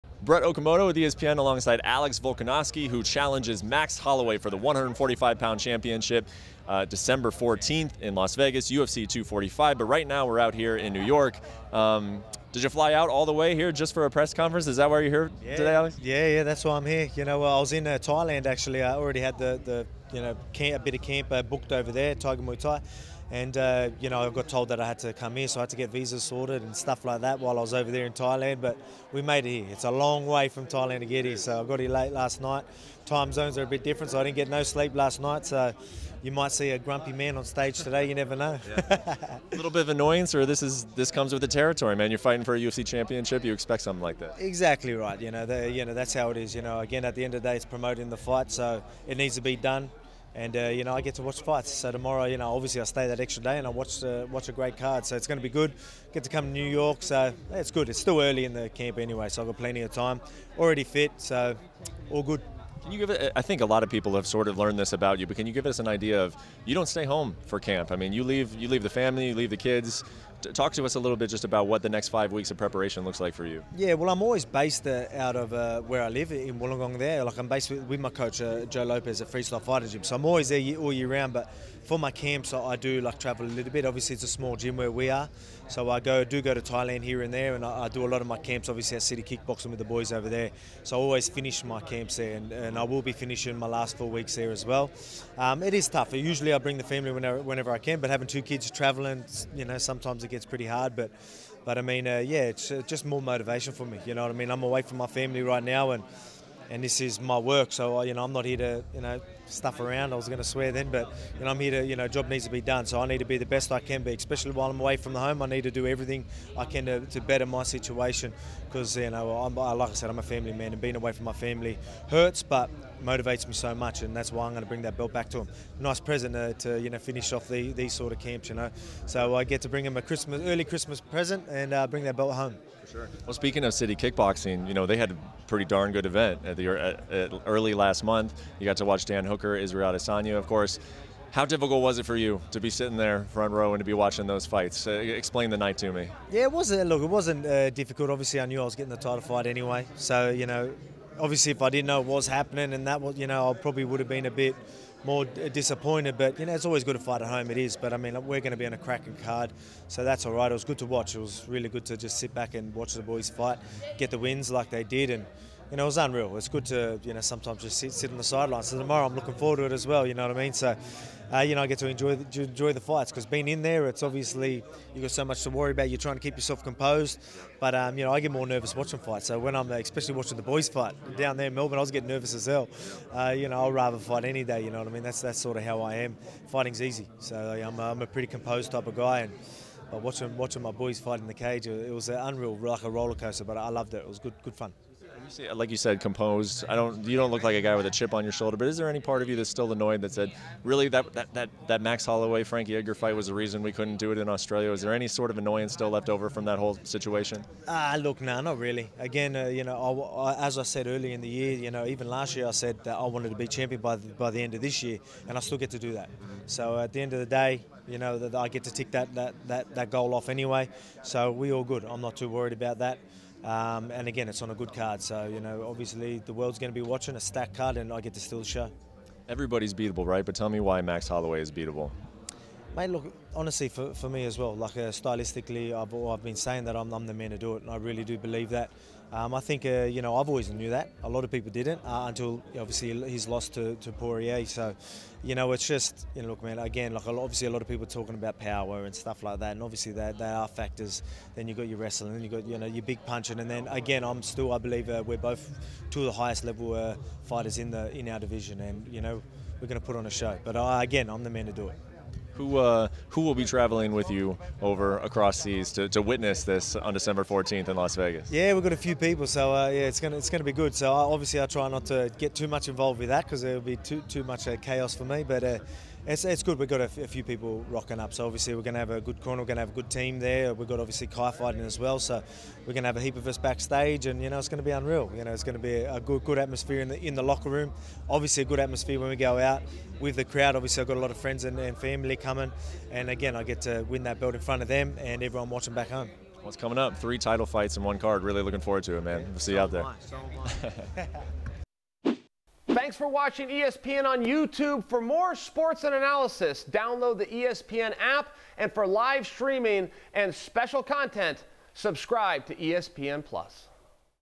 Alex Volkanovski: I’m the man to beat Max Holloway | UFC 245 | ESPN MMA
ESPN MMA’s Brett Okamoto speaks with Alex Volkanovski ahead of his UFC 245 featherweight title fight vs. Max Holloway. Volkanovski describes the nerves he had watching his City Kickboxing teammates compete at UFC 243 in Australia, then says he’s the man to beat Holloway.